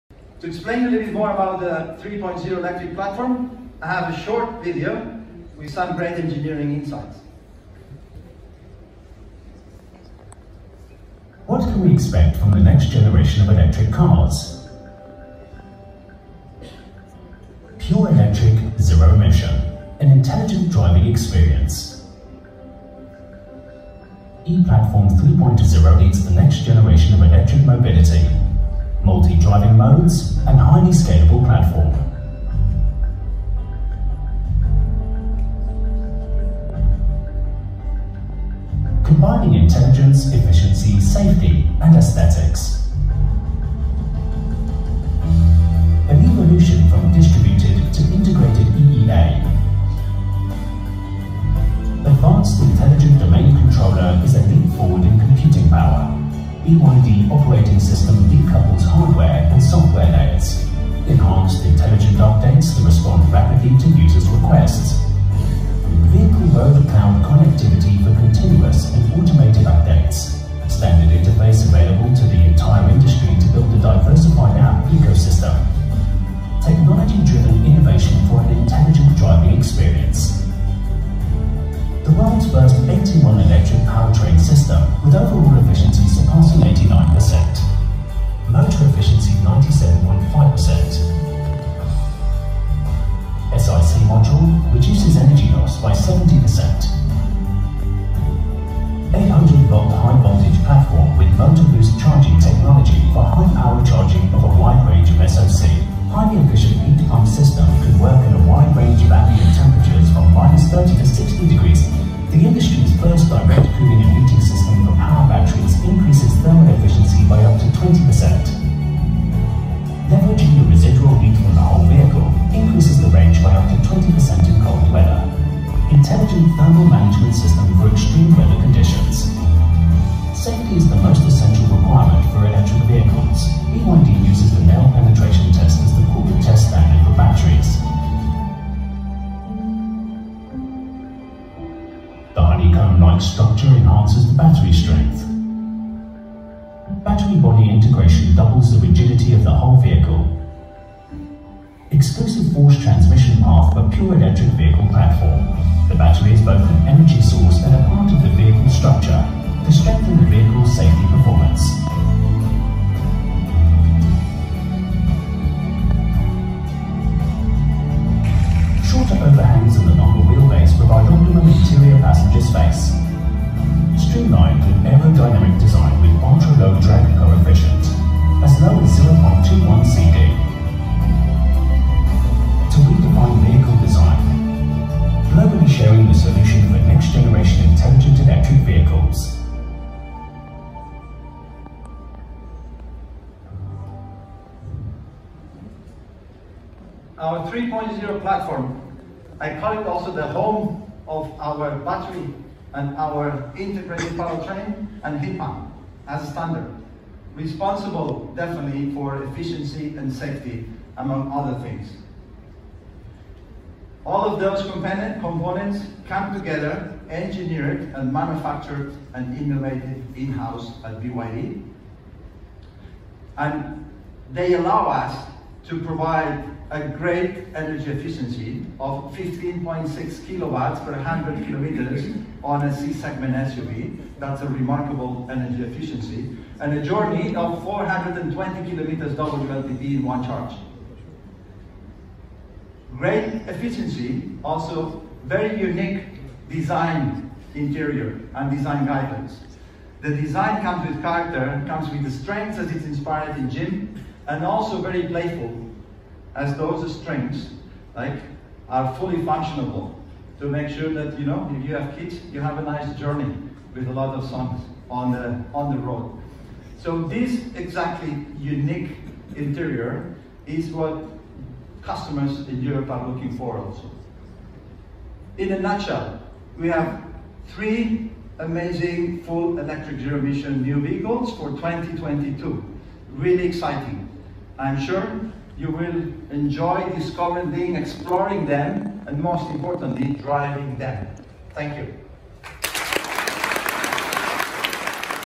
The new e-Platform 3.0 for BYD electric cars - Electric Motor News n° 33 (2022)
BYD Passenger Car Director Pere Brugal displayed a video about the new e-Platform 3.0 that is been used to build the new range of BYD electric vehicles to the European market.
In italiano - La nuova e-Platform 3.0 per le auto elettriche BYD
Pere Brugal, Passenger Car Director  BYD, ha mostrato un video sulla nuova piattaforma e-Platform 3.0 che è stata utilizzata per costruire la nuova gamma di veicoli elettrici BYD per il mercato europeo.
Video in lingua inglese. Servizio di Aurora Bovone, camera Dario Ruggeri.